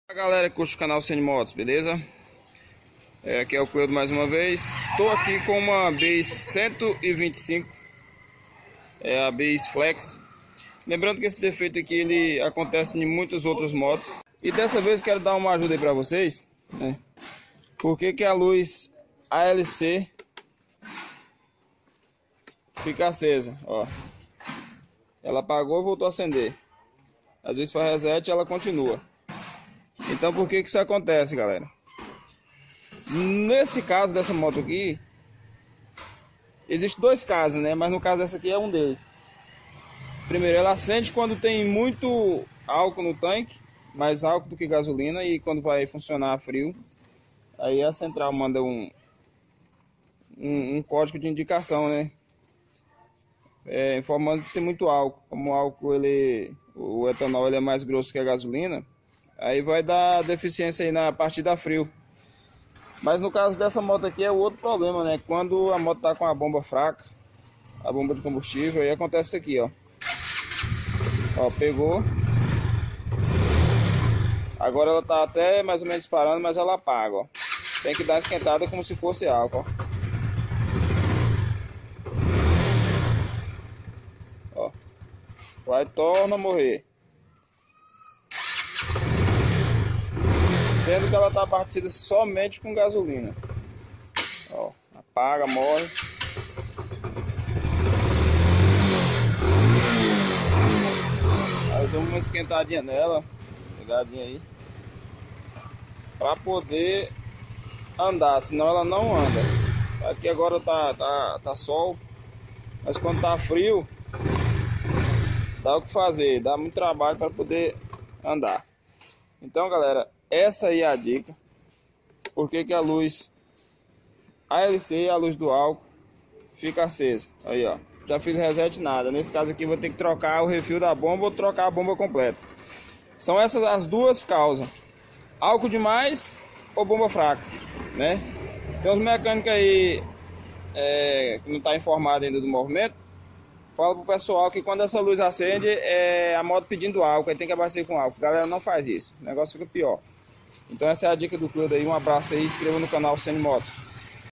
Porque a Luz ALC Acende Constantemente?! Veja as Causas e Como Apagar!!!
causa frequentes da luz ALC Acesa constantemente.
moto abastecida somente com álcool
ou bomba fraca.